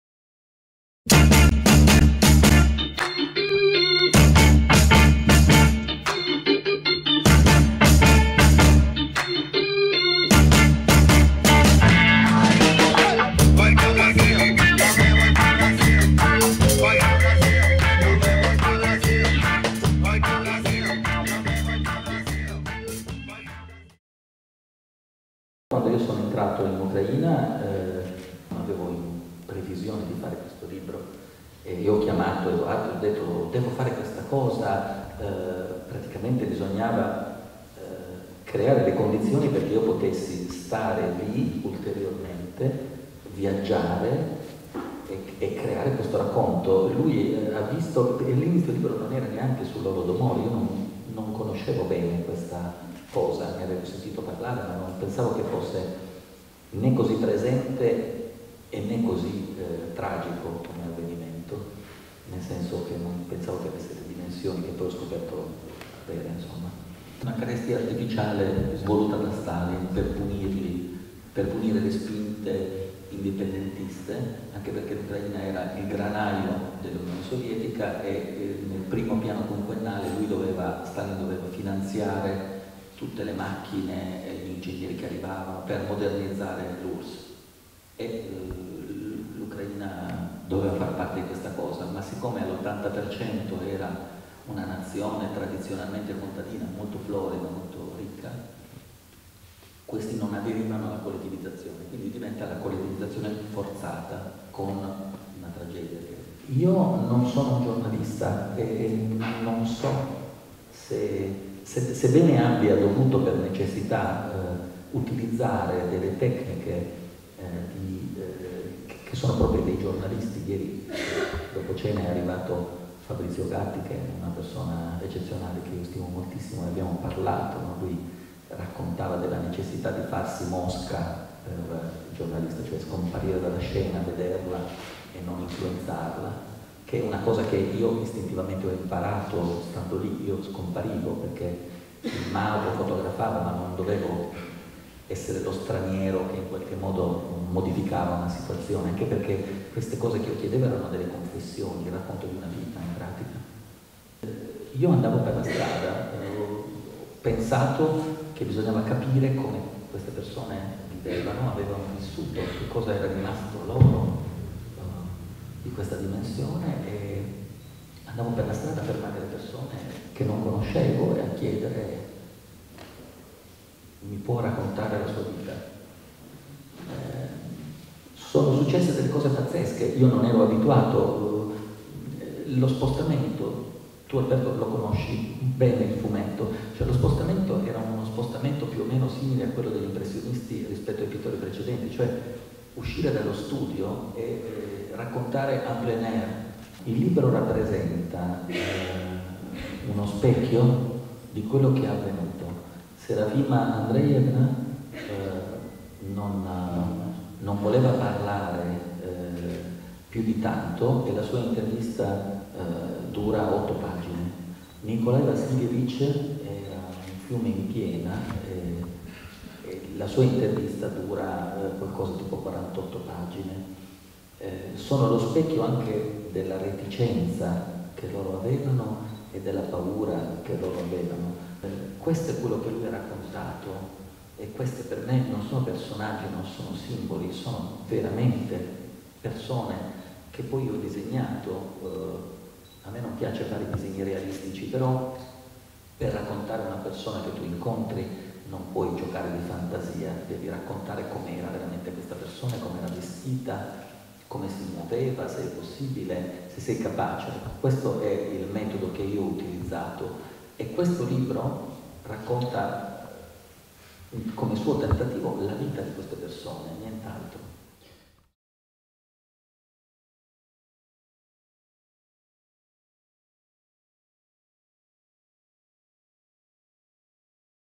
Igor Tuveri, "Quaderni ucraini"
Igort ha trascorso quasi due anni in Ucraina e Russia, e ha deciso di raccontare e illustrare le testimonianze raccolte sul periodo staliniano e le sensazioni da lui provate in questo libro, "Quaderni Ucraini". Questa la presentazione al Festivalletteratura di Mantova.